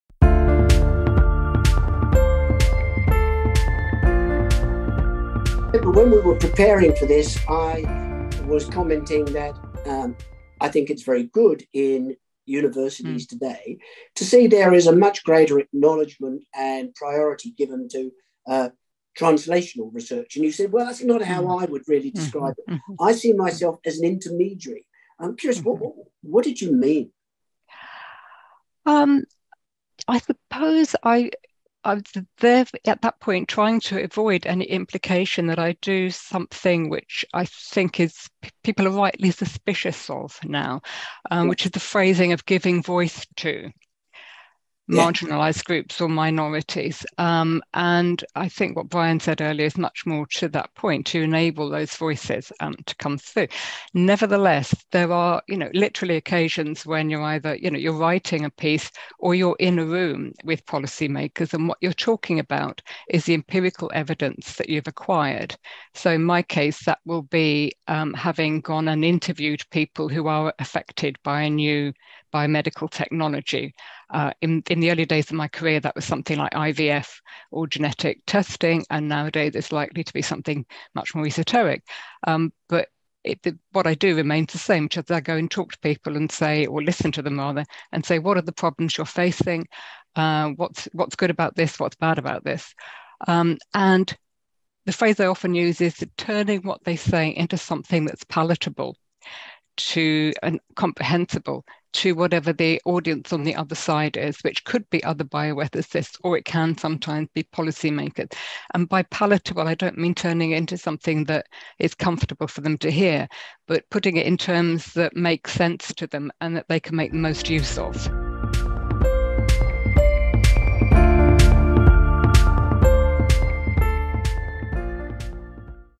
A lived experience intermediary
Professor Jackie Leach Scully explains to Professor Peter Shergold why she considers herself a lived experience 'intermediary'.

This video is an excerpt of a recording of APO's 20th Anniversary event: a panel discussion, Democratising the policy process with research and lived experience.